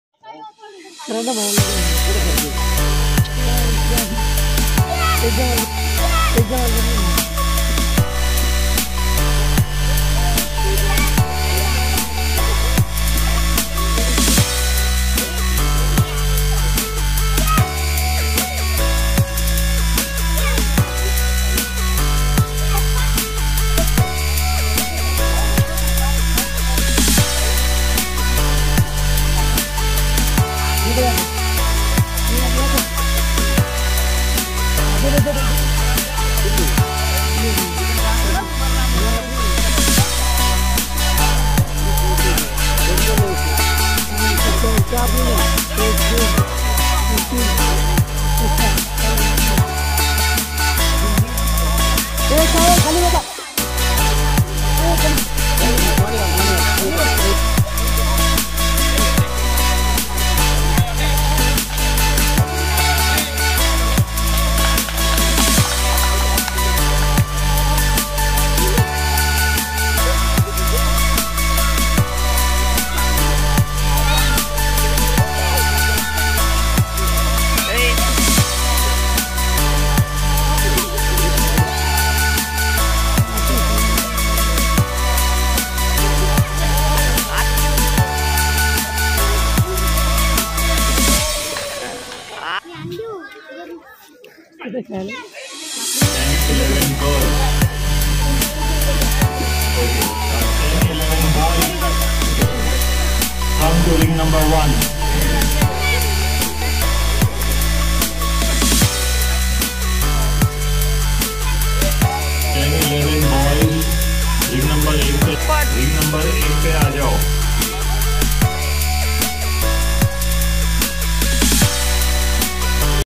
Under 7 Years Boys Kumite ||Arsalan Sayed🥋 || Kumite Final Match || State Karate Championship 2023
Arsalan Sayed Kumite || Kumite Final Match || State Karate Championship 2023